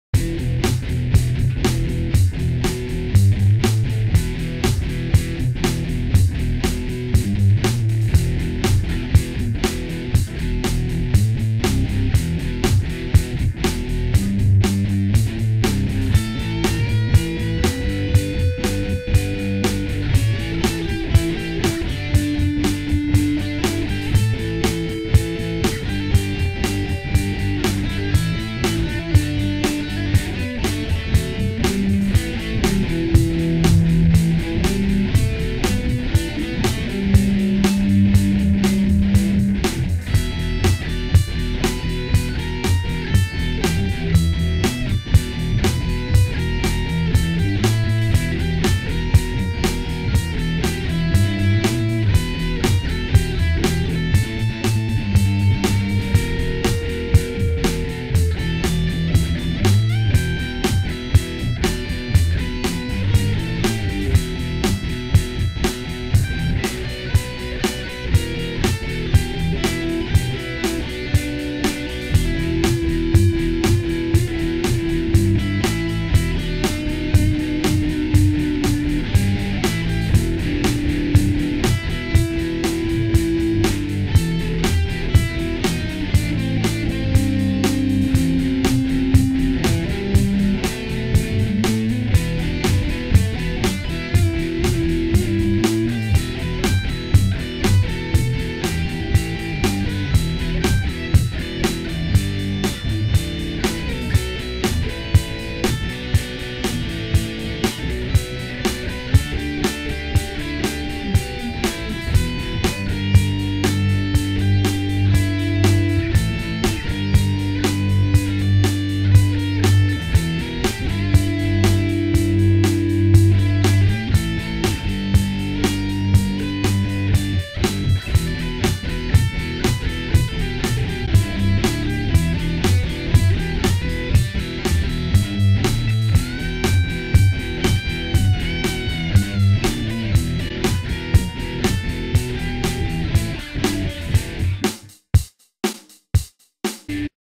mike byrne - 10_08_2011
Two distorted guitar tracks and a drum loop.